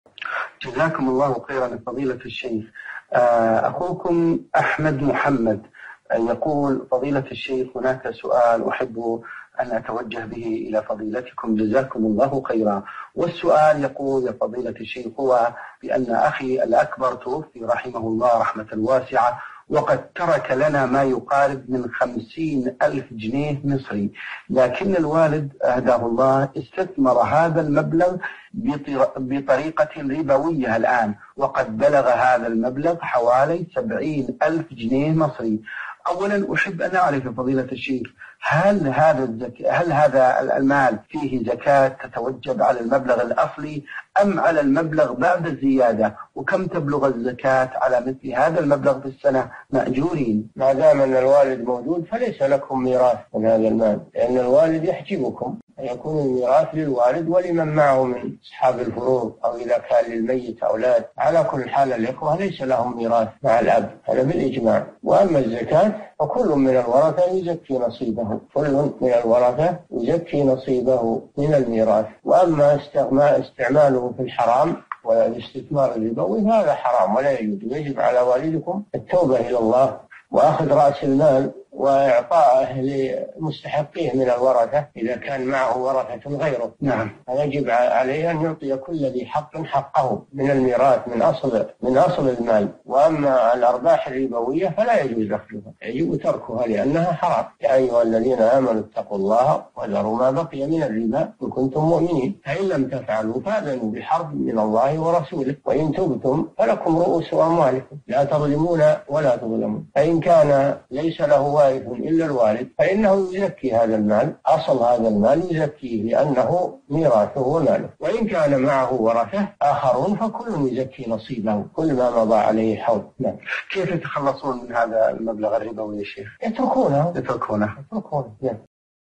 6646- هل يرث الإخوة مع وجود الأب وعلى من تجب الزكاة في مال الإرث؟ - الشيخ صالح الفوزان
"6646- أخوكم أحمد محمد يقول: فضيلة الشيخ، هناك سؤال أحب أن أتوجه به إلى فضيلتكم والسؤال يقول يا فضيلة الشيخ هو: بأن أخي الأكبر توفي رحمه الله رحمة واسعة وقد ترك لنا ما يقارب من خمسين ألف جنيه مصري لكن الوالد -هداه الله- استثمر هذا المبلغ بطريقة ربوية الآن وقد بلغ هذا المبلغ حوالي سبعين ألف جنيه مصري، أولًا: أحب أن أعرف يا فضيلة الشيخ، هل هذا المال فيه زكاة تتوجب على المبلغ الأصلي أم على المبلغ بعد الزيادة؟ وكم تبلغ الزكاة على مثل هذا المبلغ في السَّنة؟ مأجورين!
الجواب:ما دام أن الوالد موجود فليس لكم ميراث من هذا المال؛ لأن الوالد يحجبكم فيكون الميراث للوالد ولمن معه من أصحاب الفروض، أو إذا كان للميت أولاد، فعلى كل حال الإخوة ليس لهم ميراث مع الأب هذا بالإجماع.وأما الزكاة فكل من الورثة يزكي نصيبه من الميراث وأما استعماله في الحرام والاستثمار الربوي فهذا حرام ولا يجوز ويجب على والدكم التوبة إلى الله وأخذ رأس المال وإعطائه للمستحقين من الورثة إذا كان معه ورثة غيره، فيجب عليه أن يعطي كل ذي حق حقه من الميراث من أصل المال، وأما الأرباح الربوية فلا يجوز أخذها يجب تركها؛ لأنها حرام، ﴿يَاأَيُّهَا الَّذِينَ آمَنُوا اتَّقُوا اللَّهَ وَذَرُوا مَا بَقِيَ مِنَ الرِّبَا إِنْ كُنْتُمْ مُؤْمِنِينَ (278) فَإِنْ لَمْ تَفْعَلُوا فَأْذَنُوا بِحَرْبٍ مِنَ اللَّهِ وَرَسُولِهِ وَإِنْ تُبْتُمْ فَلَكُمْ رُءُوسُ أَمْوَالِكُمْ لَا تَظْلِمُونَ وَلَا تُظْلَمُونَ﴾ [البقرة: 278، 279] فإن كان ليس له وارث إلا الوالد فإنه يزكي هذا المال أصل هذا المال يزكيه؛ لأنه ميراثه وماله وإن كان معه ورثة آخرون فكل يزكي نصيبه كُلَّما مضى عليه حول.
السائل: كيف يتخلصون من هذا المبلغ الربوي يا شيخ؟
الشيخ: يتركونه، يتركونه.
السيرة الذاتية:
للاشتراك في قناة الشيخ صالح الفوزان

14_ الشيخ محمد أمين الشنقيطي
يتيوب

15_سماحة المفتي عبد العزيز آل الشيخ
 يوتيوب

رئيس الفريق
لمشروع كبار العلماء